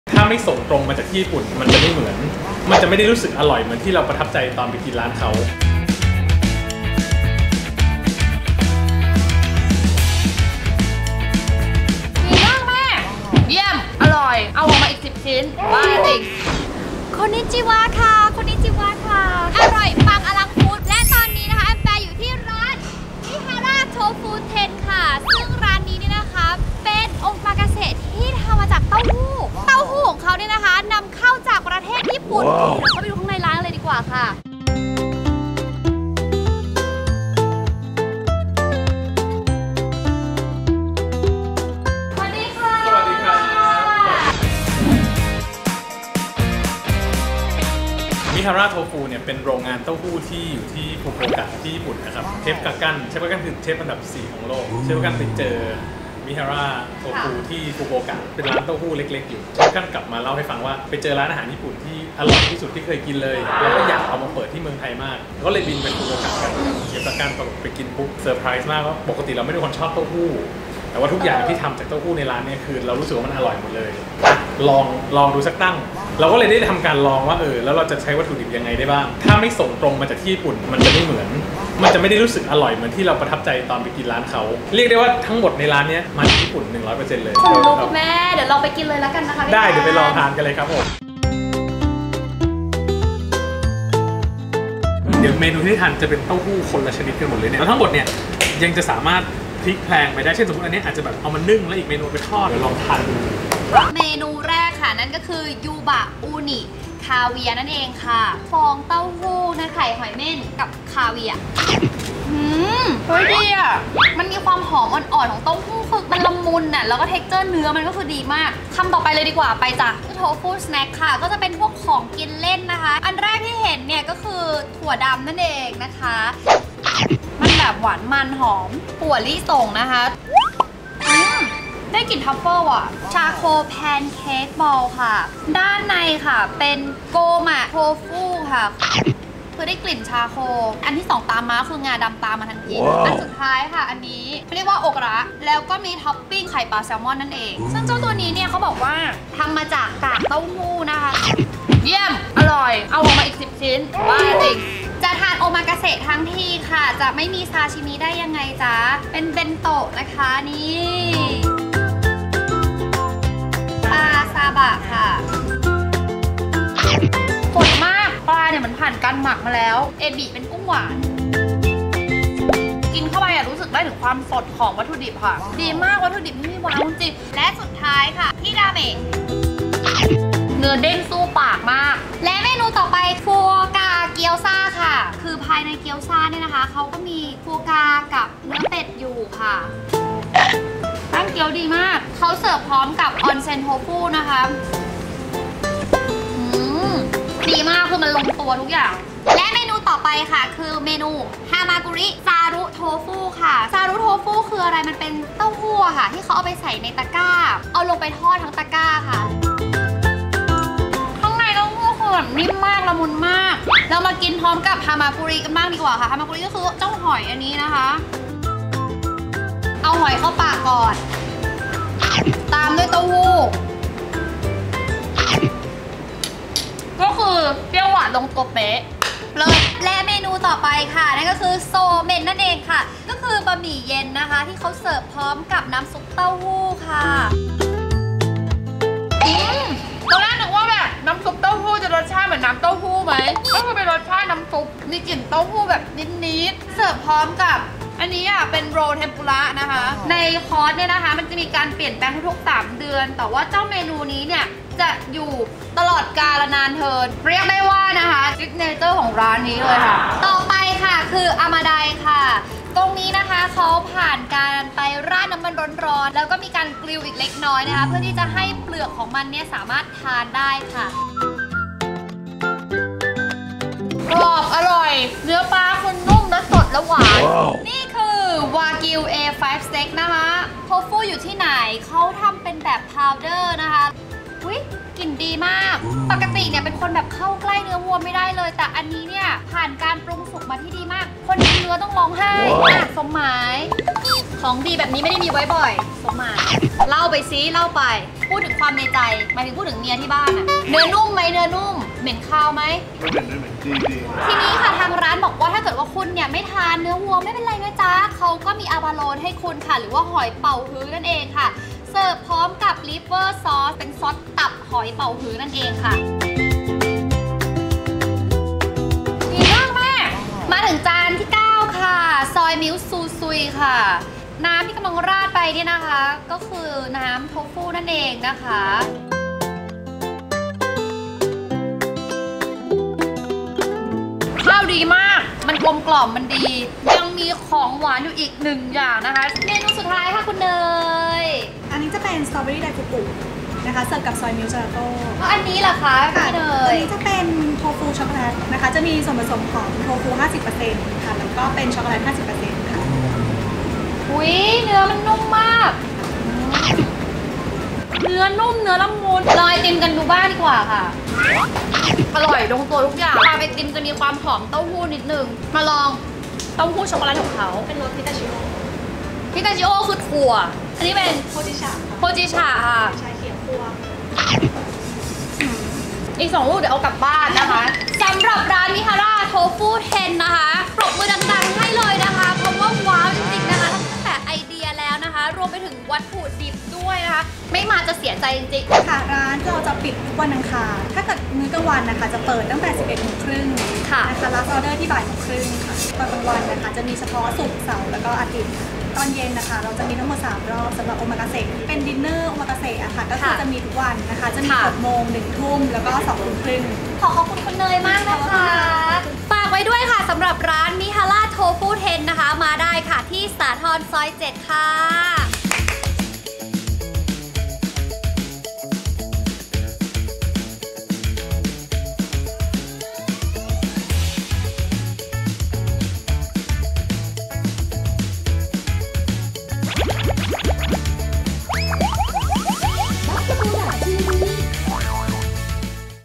อร่อยปังอลังฟู้ด EP.6 | Mihara Tofuten โอมากาเสะเต้าหู้ แห่งเดียวในโลก
Mihara Tofuten อิมพอร์ตวัตถุดิบทุกอย่างแม้กระทั่งน้ำเปล่า  ธรรมดาที่ไหนคะ
เมื่อร้าน ร้านเต้าหู้ที่มีชื่อเสียงที่สุดในฟูกูโอกะ  มาเปิดสาขาในประเทศไทย  และปรับให้เป็นสไตล์โอมากาเสะ  บอกเลยว่าสายเต้าหู้ไม่ควรพลาดจริง ๆ    ในแต่ละคอร์ดจะมีการเปลี่ยนแปลงเมนูทุก 3 เดือน

เมนูรีวิว :
Yuba uni caviar  :  ฟองเต้าหู้  ไข่หอยเม่น  กับคาเวียร์  หอมละมุน

Tofu Snacks  : เป็นเมนูทานเล่น   มีถั่วดำ   ถั่วลิสง   ชาโคลแพนเค้กบอล ด้านในเป็นโกมะโทฟุ  และอิกุระ ท็อปปิ้งด้วยไข่ปลาแซลมอน  อิกุระเป็นเมนูที่ทำมาจากกากเต้าหู้  รสชาติอร่อยค่ะ

Sashimi Bento  :   ในเซตก็จะมี ปลาซาบะ เอบิกุ้งหวาน  และฮิราเมะ

Foie Gras Gyoza  &  Onsen Tofu  :    เป็นเกี๊ยวซ๋าที่ด้านในตัวเกี๊ยวซ่า จะมีฟัวกราส์กับเนื้อเป็ด

Hamaguri Zaur Tofu :  เป็นเมนู เต้าหู้ที่แบบที่นำไปใส่ในตะกร้า  แล้วก็ทอดเสิร์ฟพร้อมกับหอย

Somen :  คือบะหมี่เย็นที่เสิร์ฟพร้อมกับซุปเต้าหู้ และโรลเทมปุระ

และเมนู Amadai ,Wagyu A5 Steak  , Abalone  , Soy Milk Zousui  ,Strawberry daifuku /Soy  milk  gelato